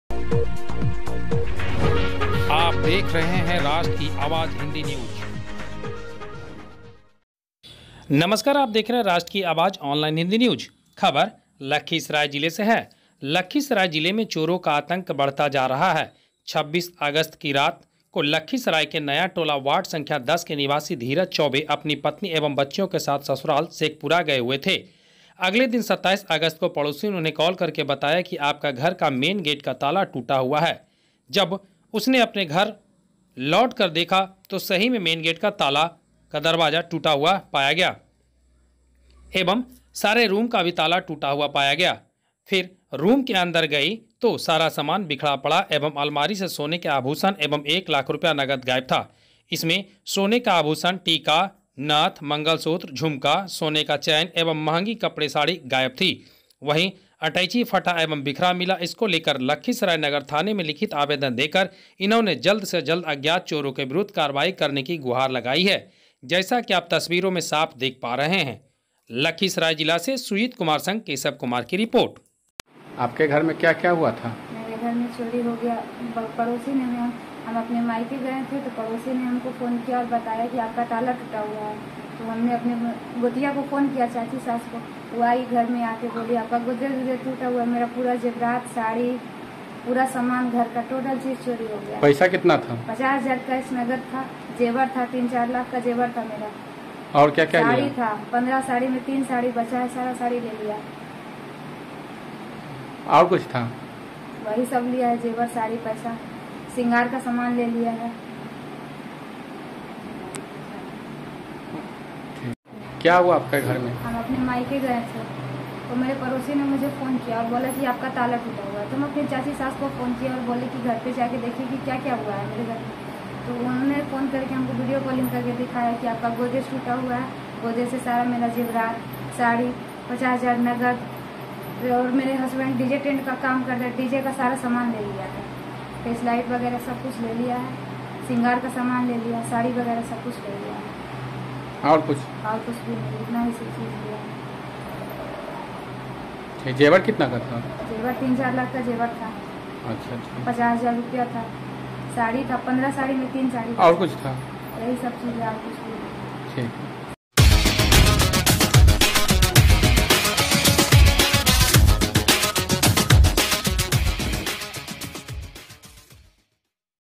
लखीसराय के नया टोला वार्ड संख्या 10 में चोरों ने सोने के आभूषण संग कपड़े पर किया हाथ साफ।
लखीसराय जिला से सुजीत संग केशव कुमार की रिपोर्ट।

लखीसराय जिले में चोरों का आतंक बढ़ता जा रहा है।  26 अगस्त के रात को लखीसराय के नया टोला वार्ड संख्या 10 के निवासी धीरज चौबे अपनी पत्नी एवं बच्चों के साथ ससुराल शेखपुरा गए हुए थे, अगले दिन 27 अगस्त को पड़ोसी ने उन्हें कॉल करके बताया कि आपका घर का मेंन गेट का ताला टूटा हुआ है जब हम अपने घर लौटे तो देखा की सही में मेन गेट का दरवाजा का ताला टूटा हुआ है एवं सारे रूम का भी ताला टूटा हुआ है फिर रूम के अंदर गए तो सारा सामान बिखरा पड़ा है एवं अलमीरा से सोने के आभूषण एवं 1 लाख नगद रुपए गायब हैं । इसमें सोने का आभूषण, टीका, नाथ, मंगलसूत्र, झुमका,सोने का चैन एवं महंगी कपड़े साड़ी गायब हैं। वही अटैची फटा एवं बिखरा मिला। इसको लेकर लखीसराय नगर थाने में लिखित आवेदन देकर जल्द से जल्द अज्ञात चोरों के विरुद्ध कार्रवाई करने की गुहार लगाई है जैसा कि आप तस्वीरों में देख पा रहे हैं।